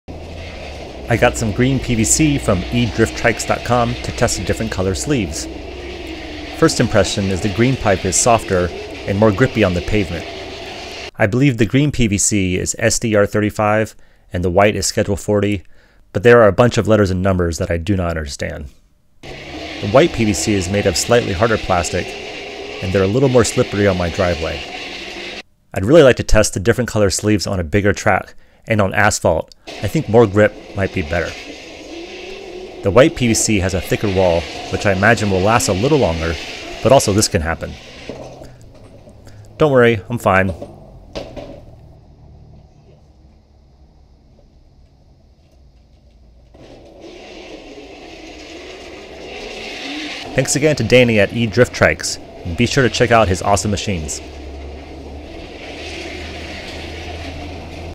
Green vs White PVC Sleeves
I got some green PVC from EDriftTrikes.com to test the different color sleeves. First impression is the green pipe is softer and more grippy on the pavement. I believe the green PVC is SDR 35 and the white is Schedule 40, but there are a bunch of letters and numbers I do not understand. The white PVC is made of slightly harder plastic and they are a little more slippery on my driveway. I'd really like to test the different color sleeves on a bigger track and on asphalt, I think more grip might be better. The white PVC has a thicker wall, which I imagine will last a little longer, but also this can happen. Don't worry I'm fine. Thanks again to Danny at @EDriftTrikes Be sure to check out his awesome machines.